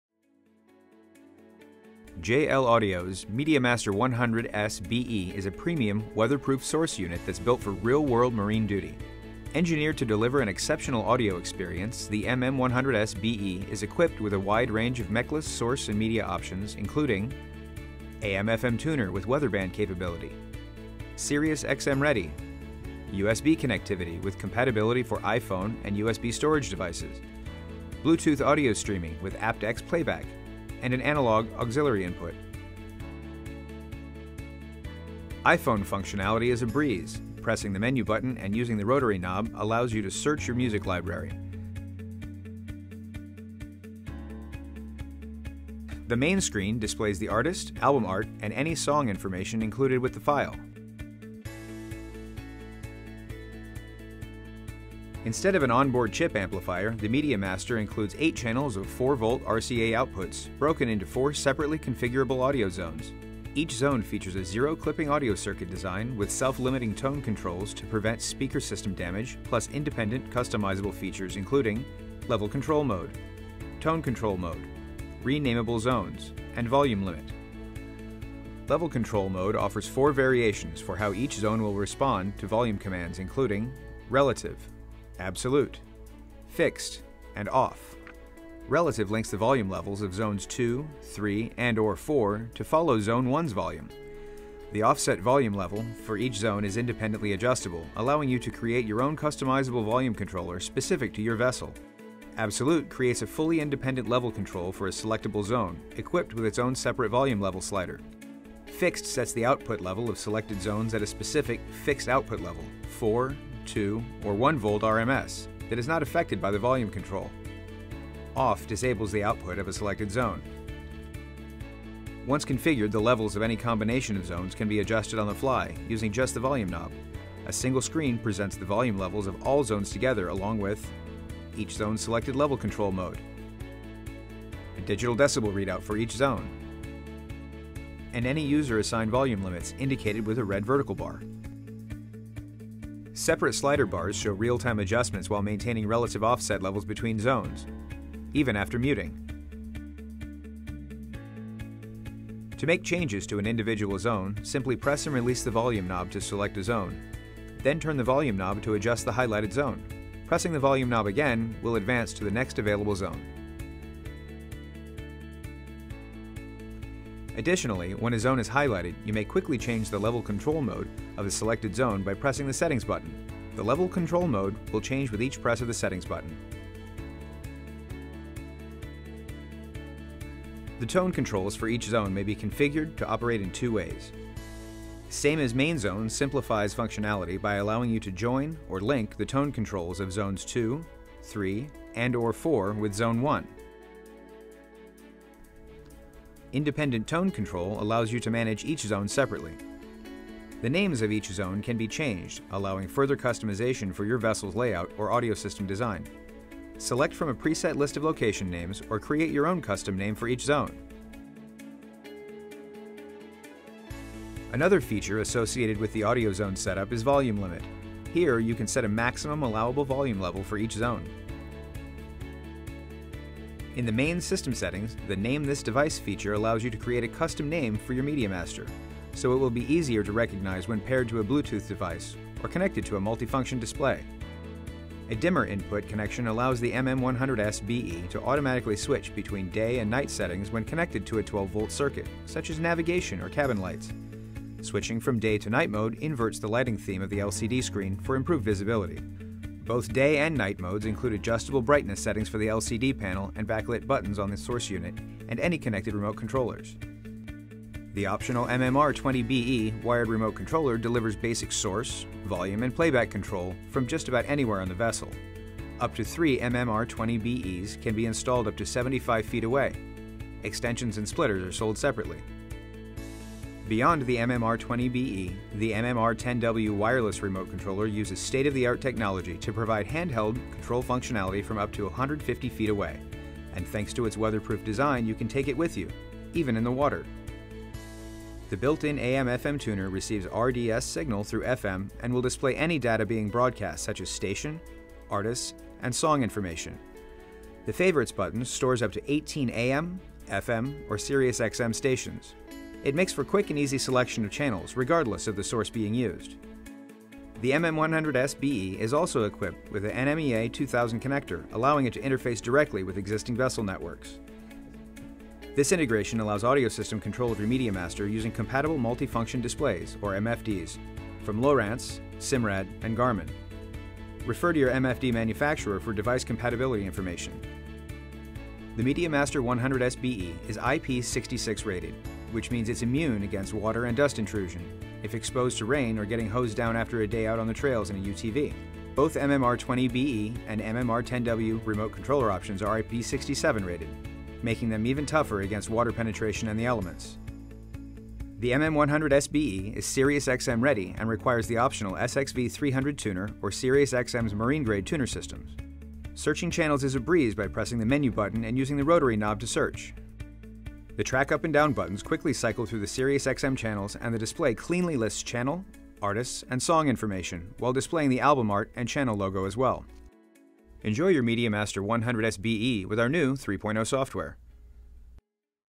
JL Audio MediaMaster 100s Black Edition Product Spotlight
Full-featured, weatherproof marine source unit with full color LCD display. The perfect solution for Marine Audio and Powersports applications. Try one on your boat, UTV, SxS or golf cart.